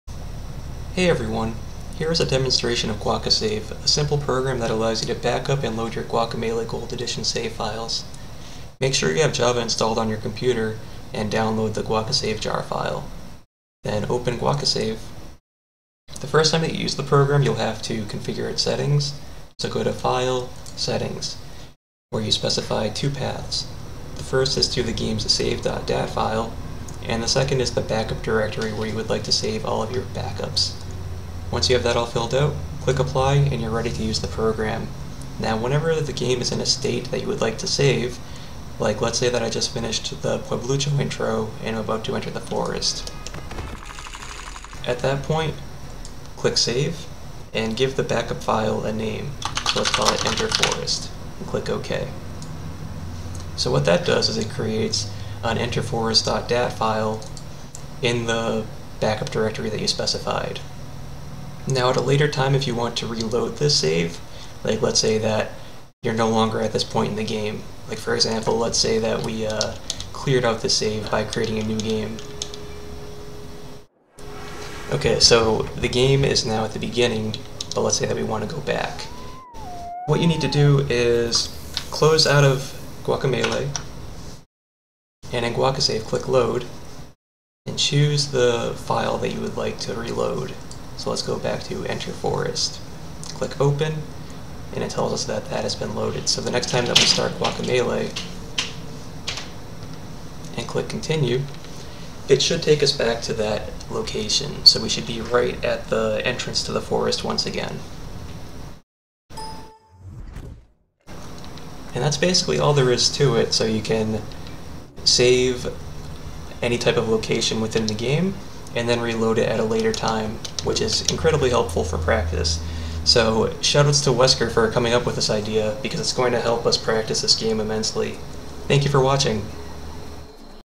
Guacamelee! GE - GuacaSave: Game Save Manager
Use GuacaSave to backup and load game saves for Guacamelee! Gold Edition.

GuacaSave is in early development, so please report any bugs and request features on the GitHub page!